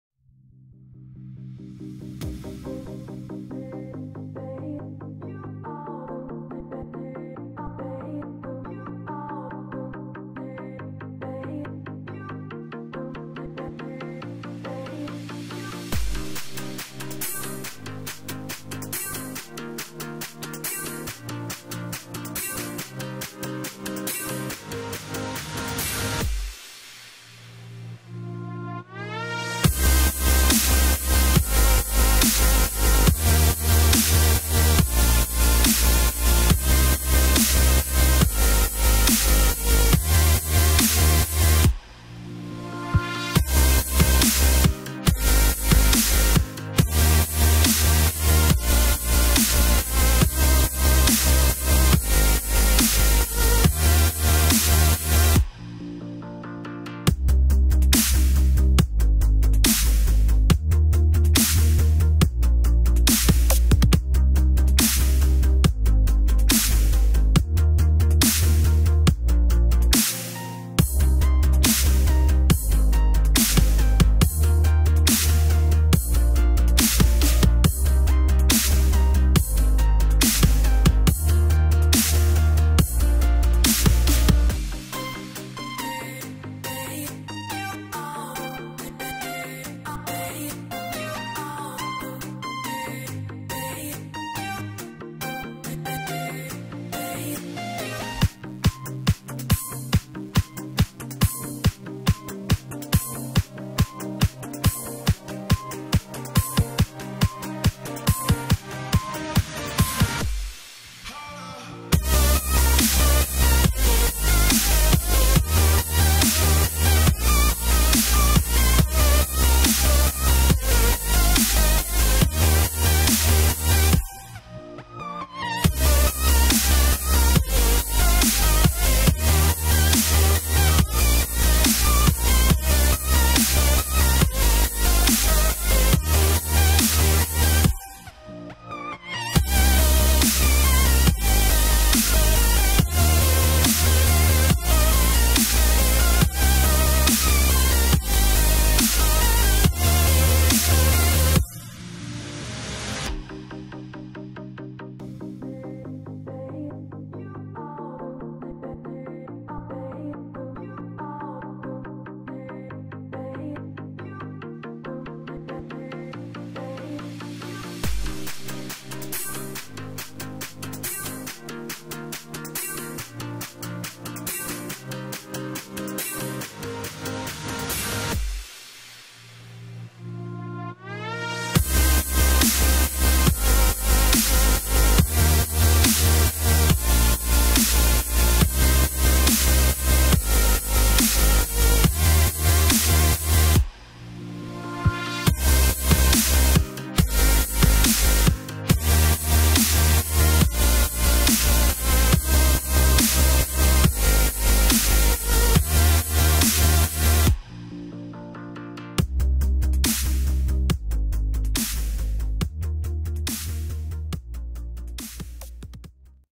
Speed Painting -Materic - Mixed media - Quadri materici - Arte moderna - Quadro In to th' Europa
REALIZZAZIONE DELL'OPERA In to th 'Europa con materiali da recupero